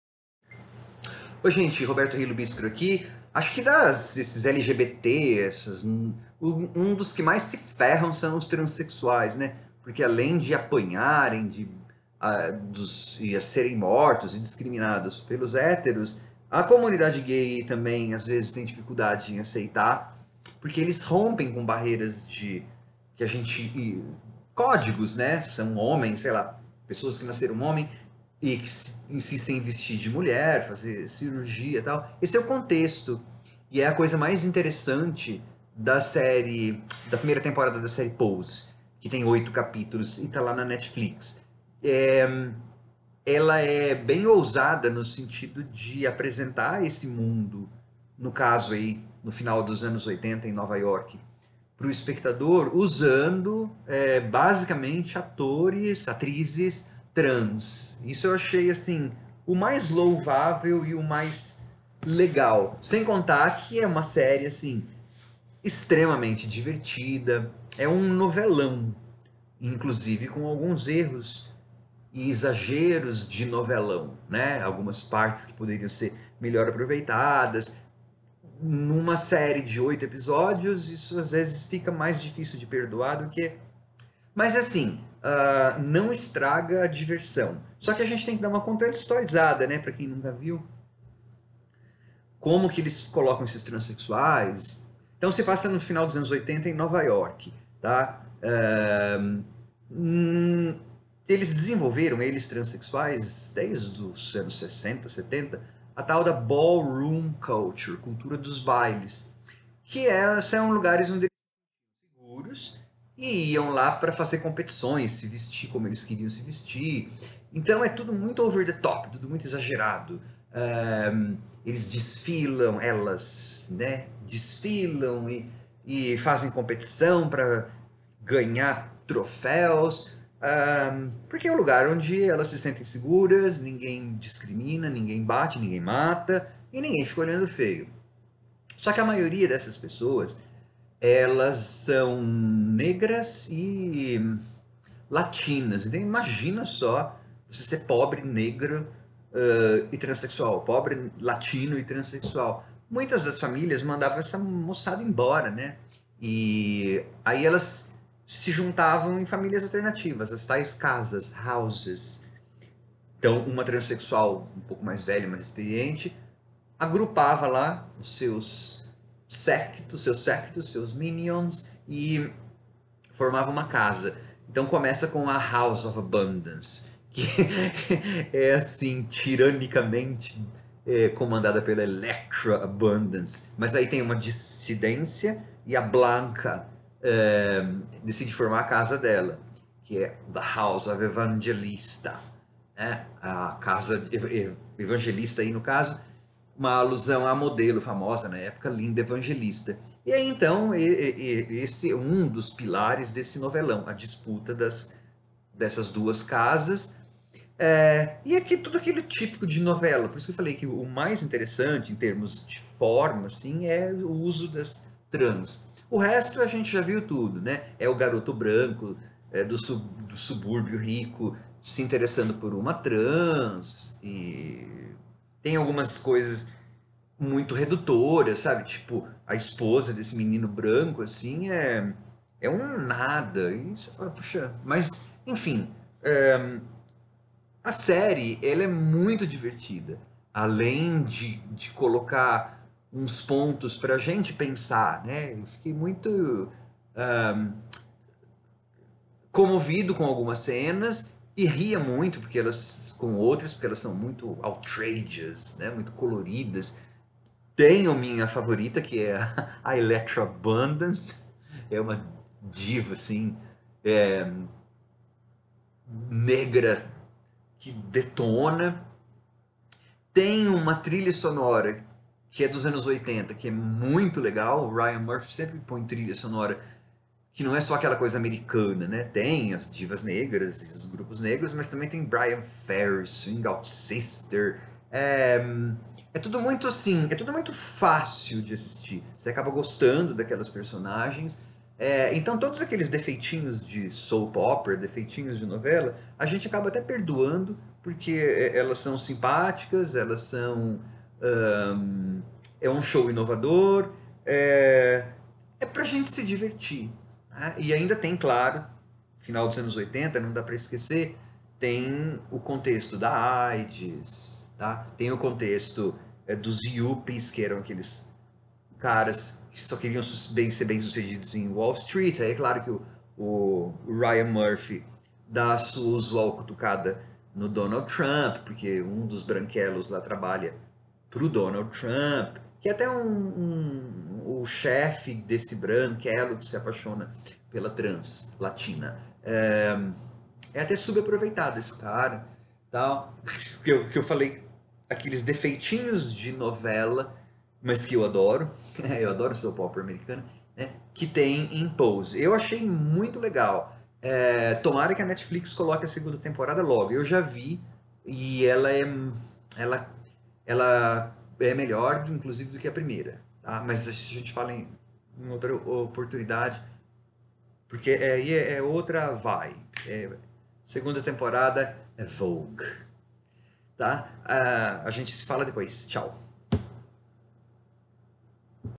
Crítica da primeira temporada de Pose (Netflix)
Em 1987 em Nova York, Blanca, uma frequentadora de bailes LGBTQ, acolhe como filhos um dançarino talentoso e uma trabalhadora do sexo apaixonada por um cliente yuppie.